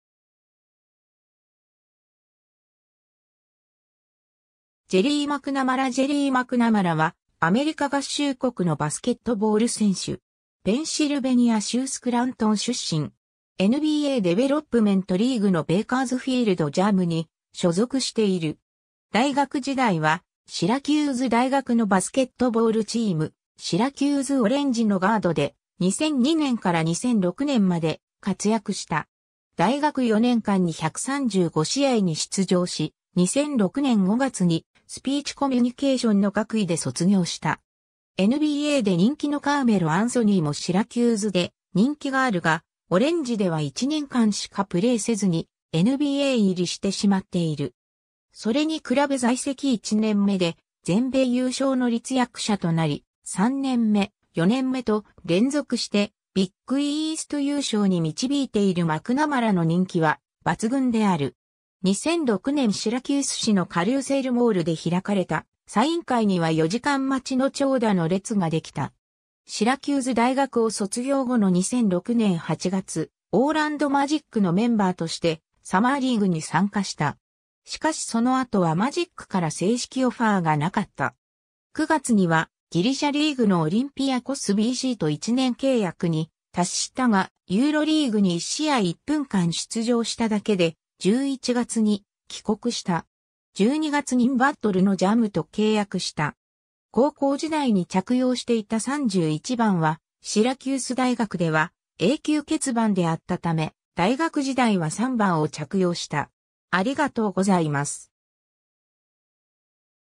ジェリー・マクナマラ
ジェリー・マクナマラ ジェリー・マクナマラ（Gerry McNamara, 1983年8月28日 - ）は、アメリカ合衆国のバスケットボール選手。
ペンシルベニア州スクラントン出身。
NBAデベロップメント・リーグ（以下NBADL）のベーカーズフィールド・ジャムに所属している。
大学時代はシラキューズ大学のバスケットボールチーム「シラキューズ・オレンジ」のガードで、2002年から2006年まで活躍した。
 大学4年間に135試合に出場し、2006年5月に「Speech Communication」の学位で卒業した。
 NBAで人気のカーメロ・アンソニーもシラキューズで人気があるが、オレンジでは1年間しかプレイせずにNBA入りしてしまっている。
それに比べ在籍1年目で全米優勝の立役者となり、3年目、4年目と連続してビッグイースト優勝に導いているマクナマラの人気は抜群である。
 2006年シラキュース市のカルーセルモールで開かれたサイン会には4時間待ちの長蛇の列ができた。
 シラキューズ大学を卒業後の2006年8月、オーランド・マジックのメンバーとしてサマーリーグに参加した（夏季に開催され、主に新人選手や若手選手の実力が見極められるリーグ）。
しかしその後はマジックから正式オファーがなかった。
9月にはギリシャリーグのオリンピアコスBC（Olympiacos BC）と1年契約に達したが、ユーロリーグに1試合1分間出場しただけで、11月に帰国した。
12月にNBADLのジャムと契約した。
 高校時代に着用していた31番はシラキュース大学では永久欠番であった為、大学時代は3番を着用した。